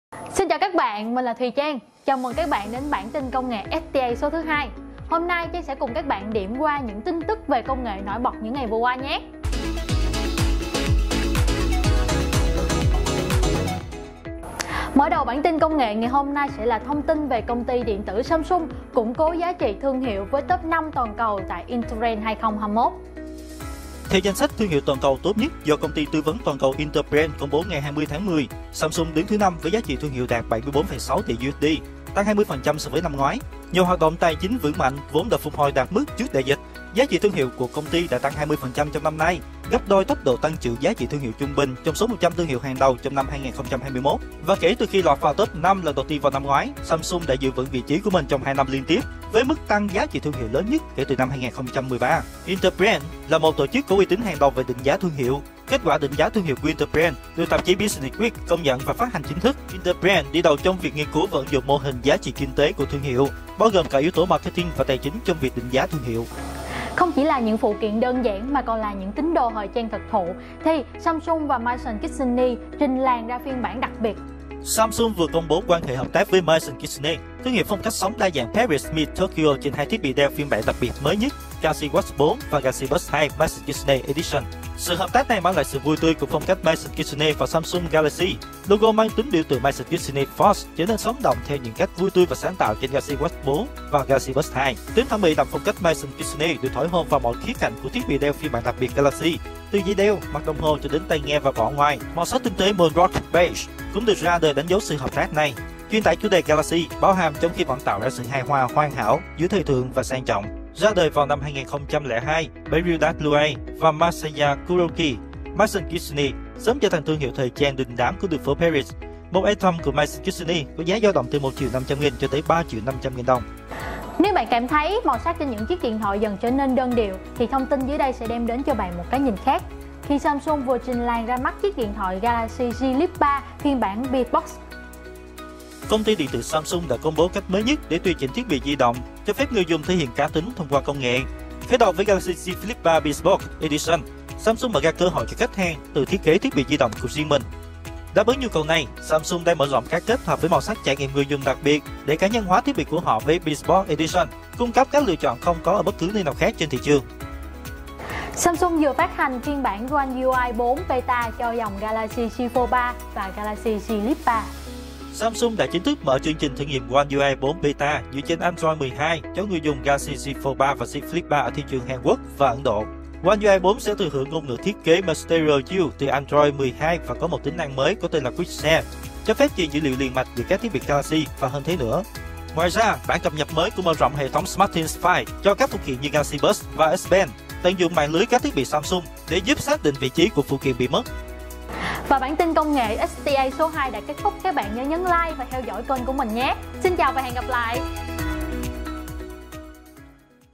Bản Tin Công Nghệ STA #2
Bản Tin Công Nghệ STA cập nhật những thông tin công nghệ mới nhất về Samsung trong tuần vừa qua:
- Samsung đạt Top 5 Thương hiệu lớn nhất toàn cầu năm 2021 theo bình chọn của Interbrand.
- Samsung và Maison Kitsuné trình làng phiên bản đặc biệt Galaxy Watch4 và Buds2.
- Trải nghiệm dòng Galaxy tùy chỉnh đột phá mới: Trình làng Galaxy Z Flip3 Bespoke Edition.
- Samsung đã chính thức phát hành phiên bản One UI 4 Beta dựa trên nền tảng Android 12 cho các thiết bị Galaxy.

© Bản quyền nội dung thuộc về Samsung Training Academy